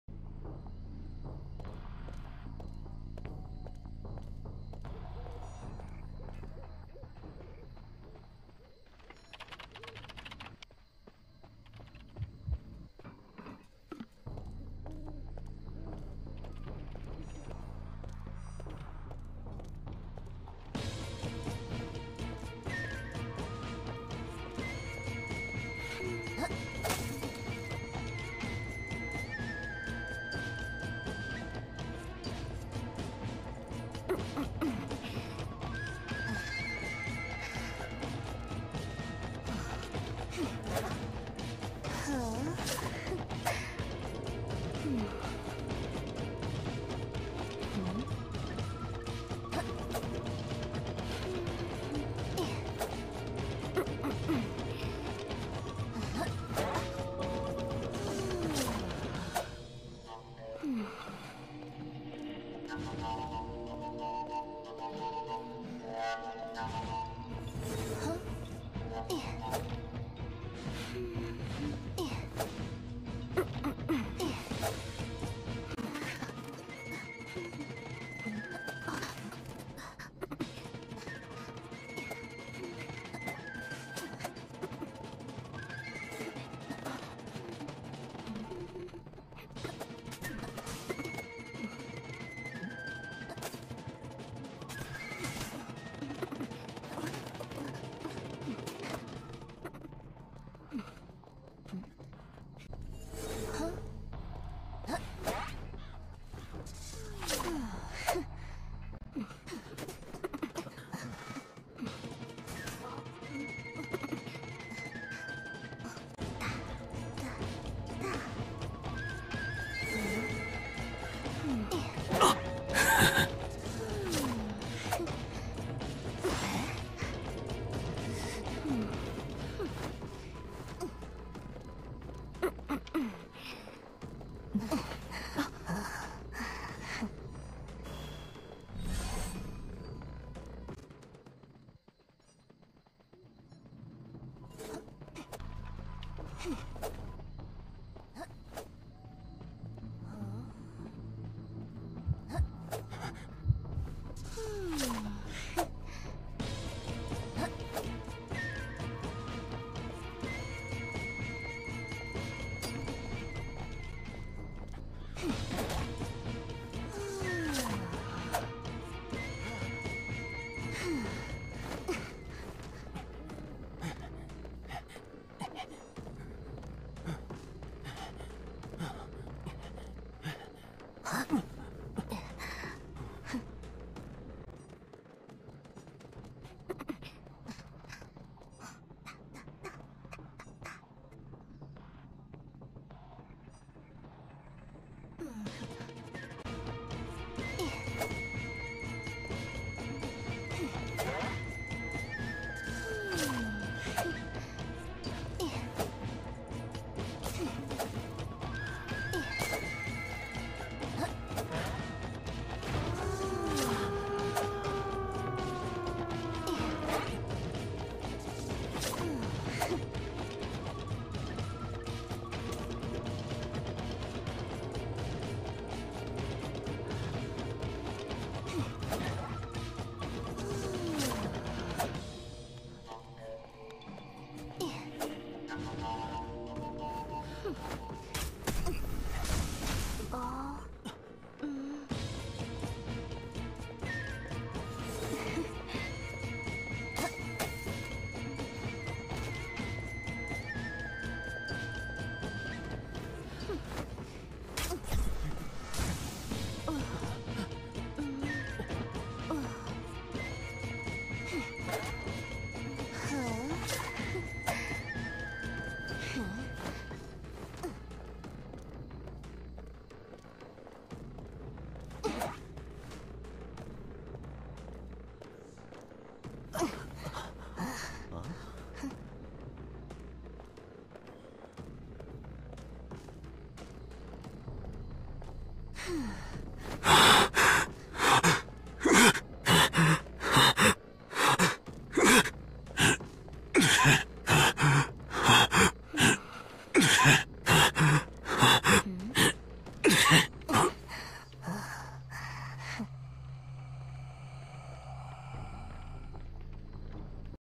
mary gameplay ;; norton is loud as hell...
totally 100% pro s badge mary gameplay
i got harassed so hard at the beginning of the match dni